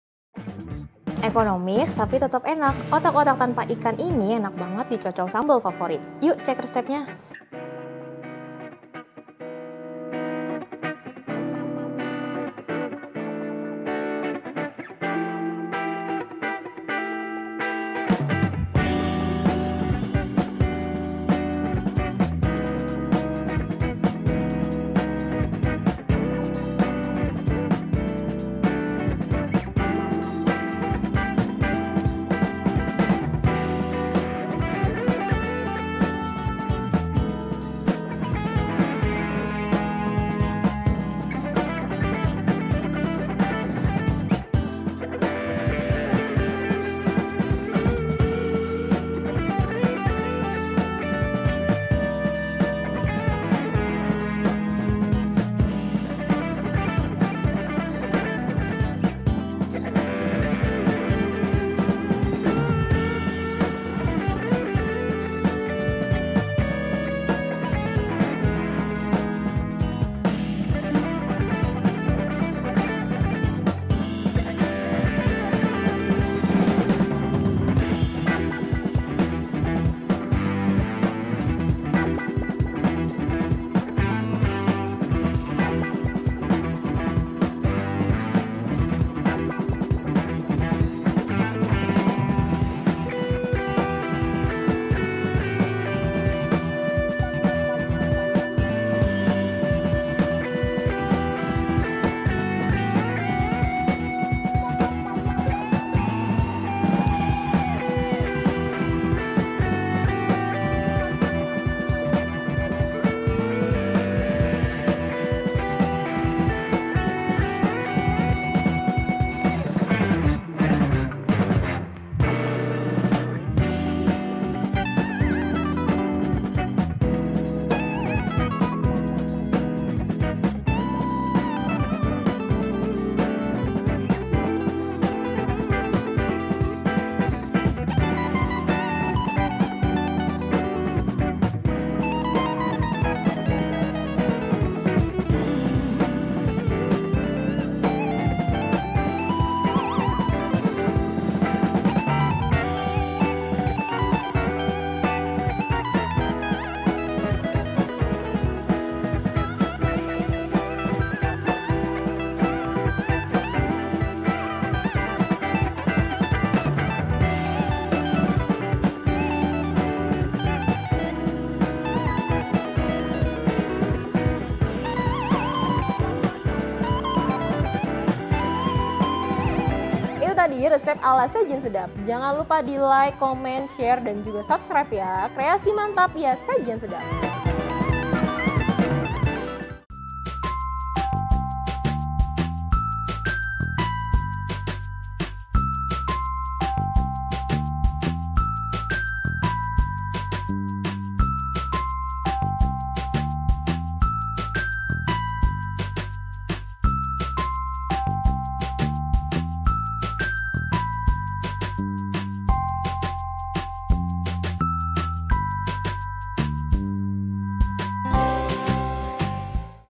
CUMA MODAL TEPUNG, Resep Otak Otak Tanpa Ikan Ini Tetep Garing Kenyal Maksimal! Wajib Coba!
Mau buat otak-otak di rumah tanpa ikan? Bisa banget. Otak-otak ini biasa dinamakan dengan otak-otak dos. rasanya tetap enak kok kayak biasanya! Cus simak videonya sampai habis.

Produser Eksekutif: Intan Yusan Septiani
Penulis Resep: Indri, Resi, Qorin, Ruth
Penulis Naskah: Rika
Peraga: Resi, Indri, Qorin, Ruth
Fotografer: Andrian, Osyad, Bram, Yudi
Videografer: Andrian, Osyad, Bram, Yudi
Video Editor: Andrian, Osyad, Bram, Yudi